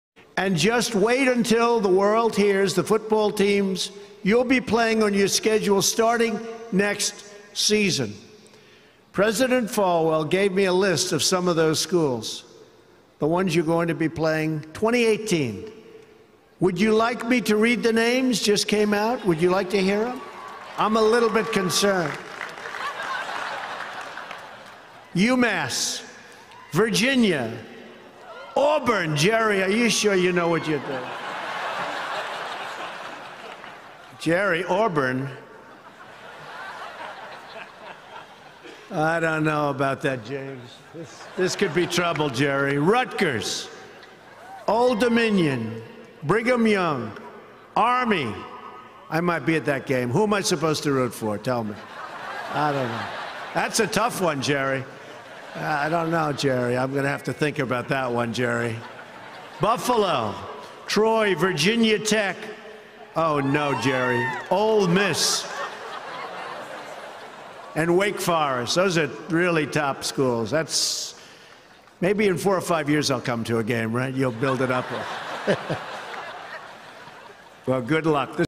President Trump reveals Liberty's future opponents during commencement speech
Liberty welcomed President Donald Trump to campus on Saturday as its commencement speaker. And while the president addressed the 2017 graduating class, he announced some football news.

Yes, really! After offering his assessment of Liberty’s upcoming jump to the FBS level, Trump, with the blessing of school president Jerry Falwell Jr., announced several future opponents for the Flames that had not yet been made public.Liberty’s two-year reclassification process was approved by the NCAA in February. The Flames will stay at the FCS level in 2017 before beginning play as an FBS independent in 2018. In 2019, Liberty will then be able to compete in FBS bowl games.

As announced by President Trump, the program’s first FBS home game will be against Old Dominion. Liberty will also have future games against Army, Auburn, BYU (2019, 2022), Ole Miss (2021), Rutgers (2019), Virginia (2018, 2019, 2027), Virginia Tech (2020) and Wake Forest (2022, 2025, 2026), among others.

“It will be a thrilling time during the 2018 season for Liberty Football,” coach Turner Gill said. “Not only are we transitioning to the FBS level, but we are playing some great teams and hope to show our readiness for the next step up. We are also excited for our fans to see the tremendous team we will be putting on the field to entertain them in 2018.”